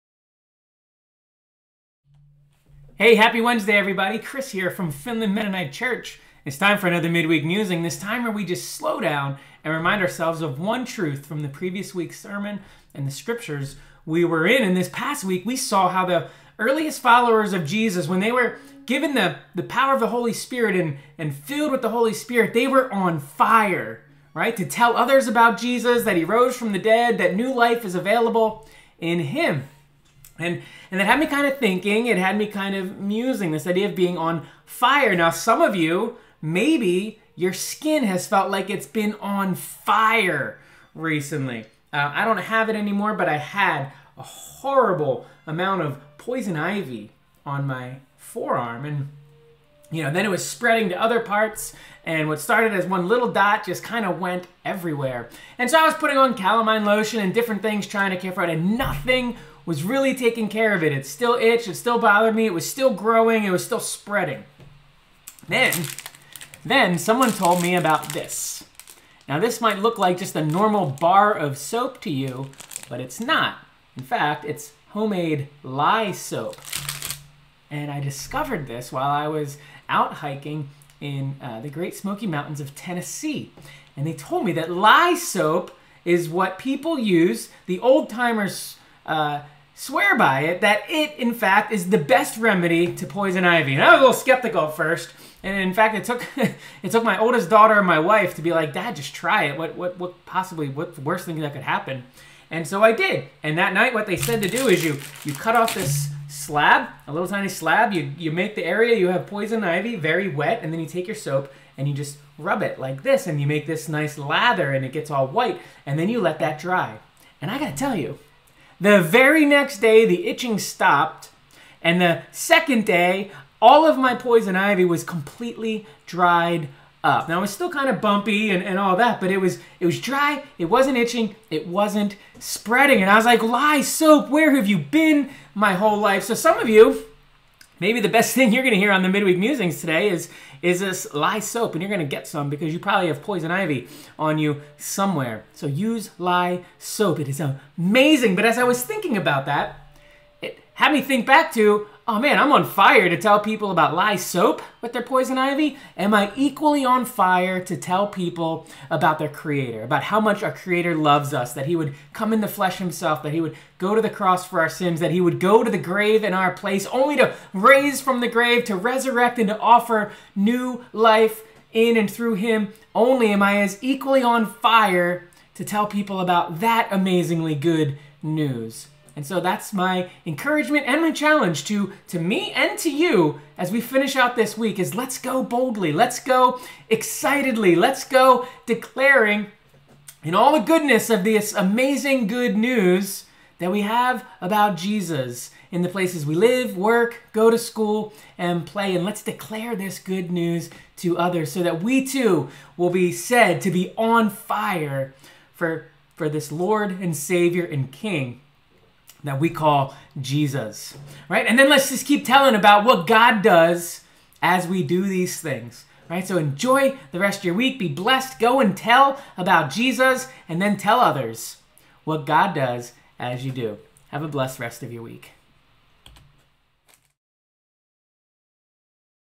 Midweek Musings "Treating Poison Ivy" | June 8, 2022
I recently got poison ivy and discovered an amazing, powerful, ultra quick treatment for it. What does this have to do with being OnFire for Jesus? Join in and find out.